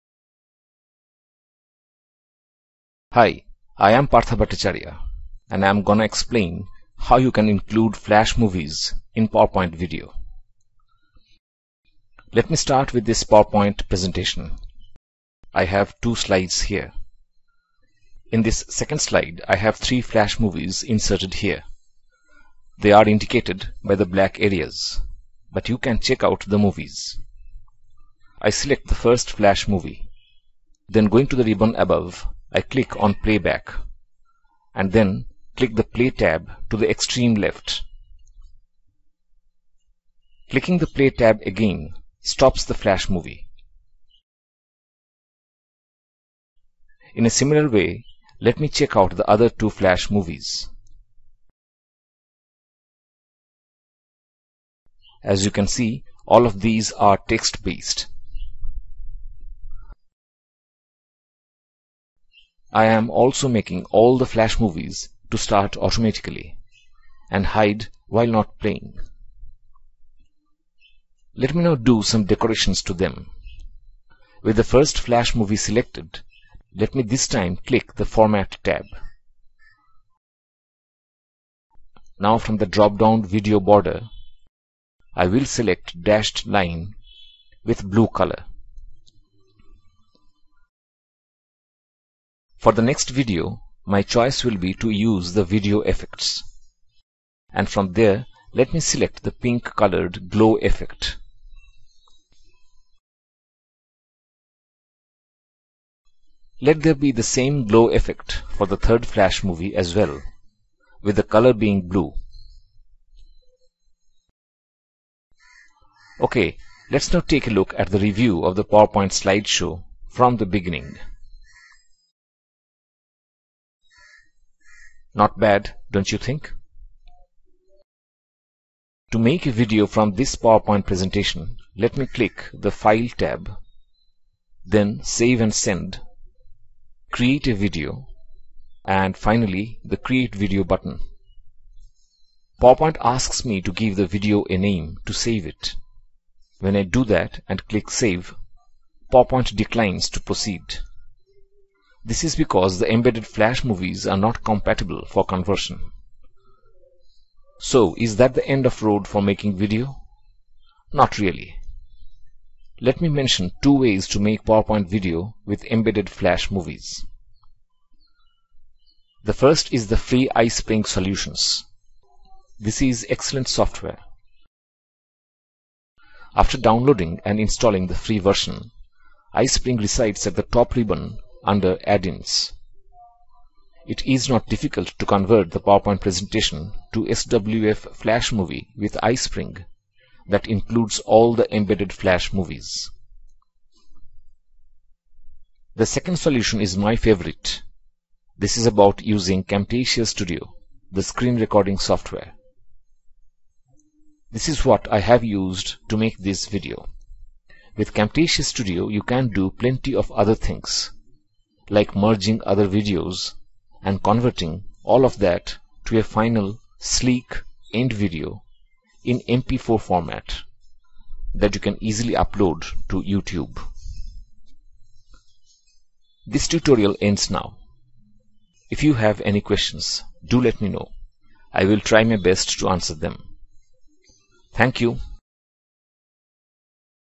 3 Flash Movies In PowerPoint Animation Video
for more video tutorials.

In PowerPoint 2010 you can embed SWF flash movie as part of presentation.

If however you want to convert the presentation into a PowerPoint video in WMV format, you cannot do that because the flash movie is not  compatible for conversion.

But there are a few other ways to ensure that your PowerPoint video does have one or more embedded flash movies.

This video explains how with the help of 2 software you can embed 3 flash movies in a single PowerPoint slide and convert them into sleek PowerPoint video.

For similar video tutorials, do visit the 2WebVideo.Com blog at the above link.